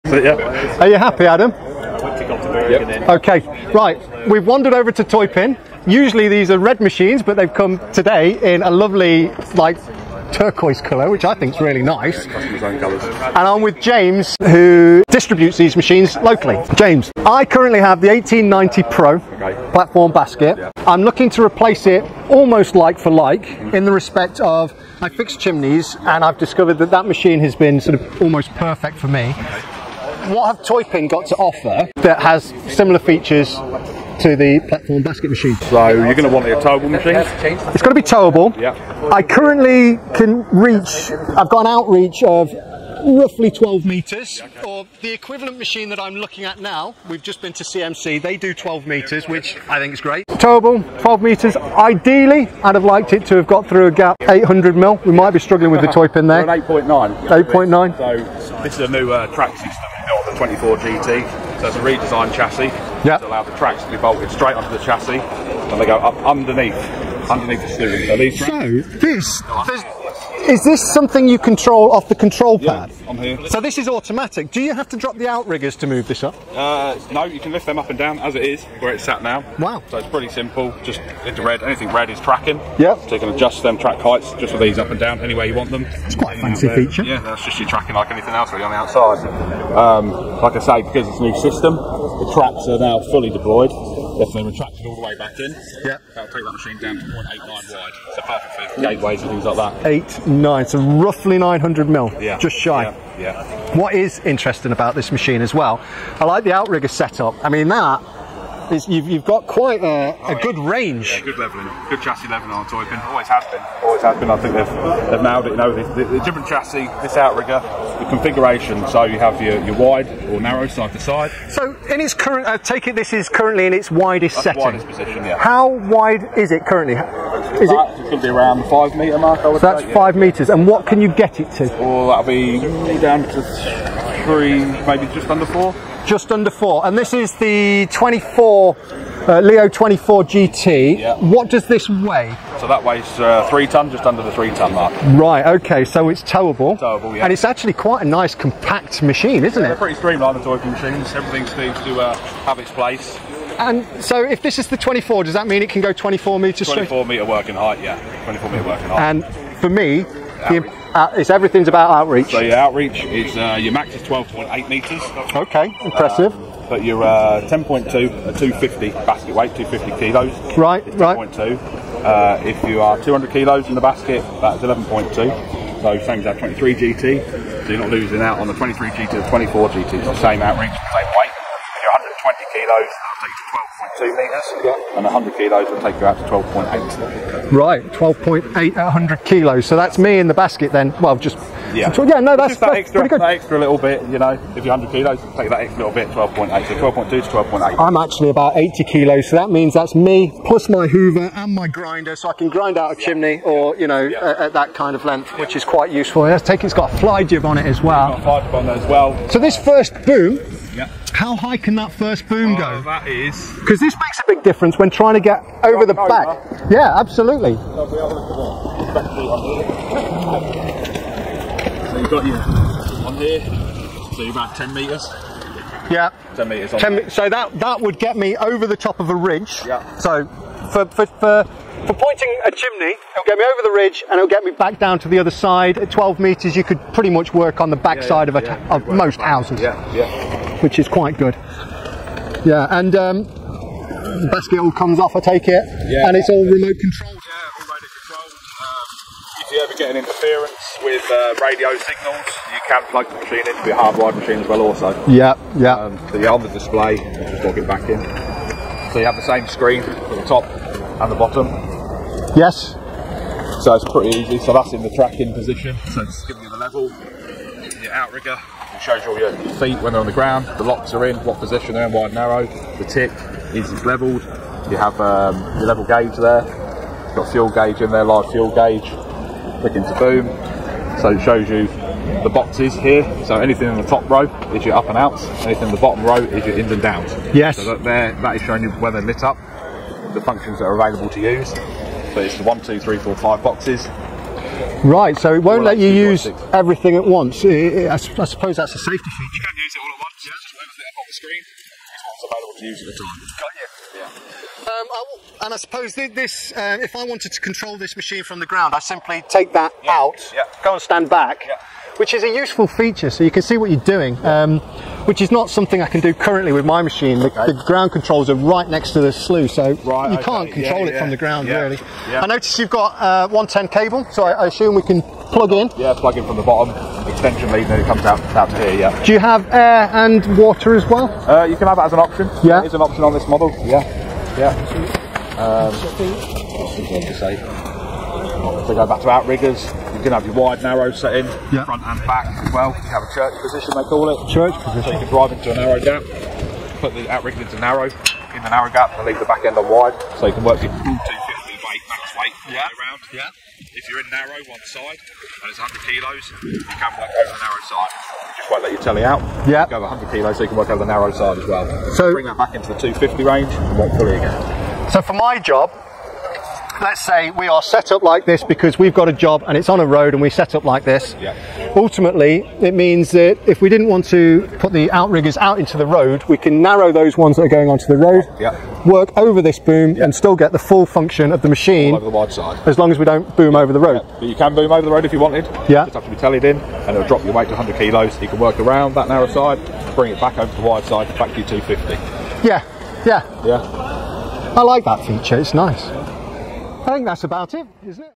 Teupen LEO 24GT - Is This the Perfect Chimney & Roofing Spider Lift?
I’m hands-on with James Skinner from Alfa access services Ltd, the local Teupen distributor, to look over the LEO 24GT in a striking turquoise finish.
I currently run a Platform Basket 1890 Pro and I’m hunting a near like-for-like replacement for chimney repairs, pointing, and tricky roof access. In this video we break down the features that matter on real jobs.

What you’ll see in this walkthrough:

Towable tracked spider lift weighing just under 3 tonnes - compact and streamlined layout.

Working height: 24 m with impressive outreach up to 12.8 m (at ~100 kg in the basket).

Real-world load chart highlights: ~10.2 m at 250 kg, ~11.2 m at 200 kg, ~12.2 m at 120 kg, up to 12.8 m at 100 kg.

Narrow access winner: redesigned 24 GT chassis with tracks that retract to about 890 mm - ideal for tight gateways and passages.

Smart outriggers: wide/narrow configurations front and rear, plus the handy “church” setup for working in lanes or beside roads without pushing legs into traffic.

Ground clearance & track height control from the pad - raise/lower tracks without dropping outriggers.

Fly jib for getting over a ridge and down the far side - brilliant for chimney pointing and back-of-house access.

Dual controls & radio remote (with hard-wire backup) so you can operate safely from the best vantage point on the ground or in the basket.

110V plug-in shore power, with optional air and water lines to the basket.

Why it matters for chimney & roofing work:
If you’re grinding, repointing, or flaunching chimneys, outreach and setup flexibility are everything. The LEO 24GT’s 10 m+ first boom rise helps clear ridgelines, while the variable outrigger footprint keeps you within kerbs when you must avoid narrowing a road. For solo or two-person teams with tools, the 250 kg basket capacity is generous without sacrificing stability.

• 🛠️ Amazon Shop

00:00 Introduction
00:31 What Has Teupen Got To Offer?
01:08 The Tracks Explained
01:43 The Outriggers
02:21 The Weight Of The Spider
02:33 How High Can It Go?
02:44 What Is It's Outreach?
03:27 The Boom Configuration
03:47 Working Envelope
04:03 The Remote Control
04:19 Machine Displays
05:06 Controlling The Machine From The Ground
05:38 Outriggers Setup